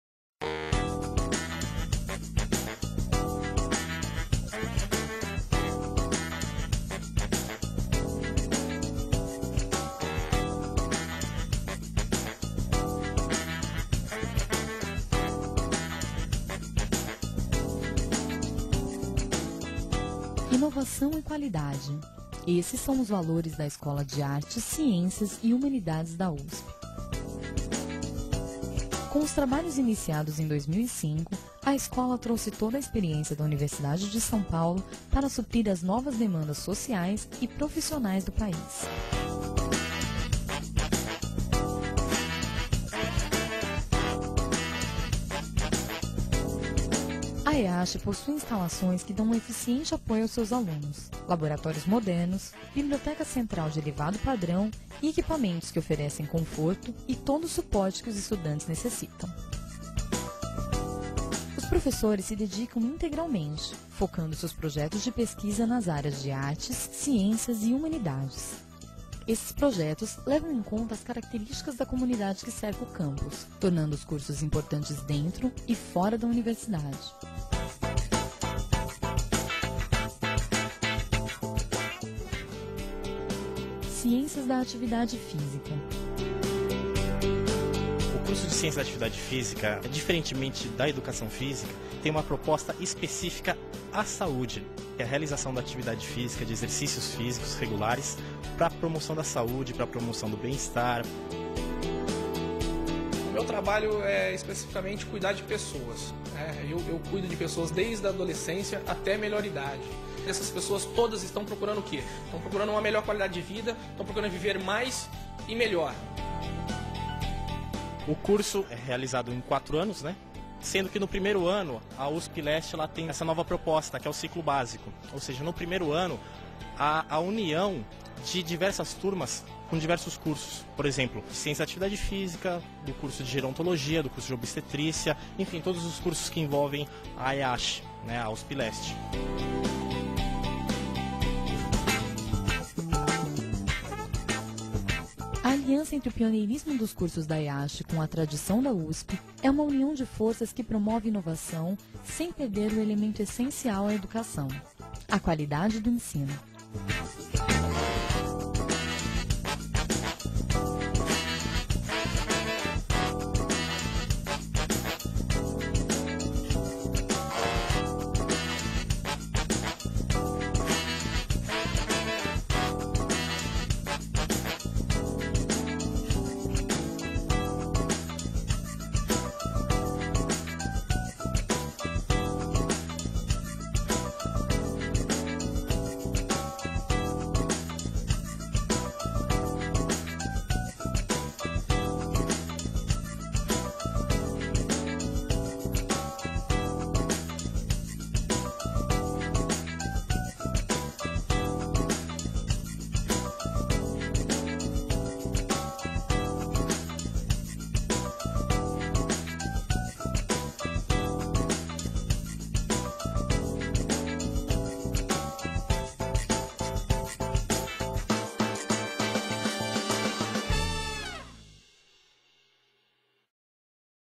Ciência da Atividade Física - Vídeo Institucional EACH
Trecho do vídeo institucional da EACH sobre o curso de graduação Bacharelado em Ciências da Atividade Física.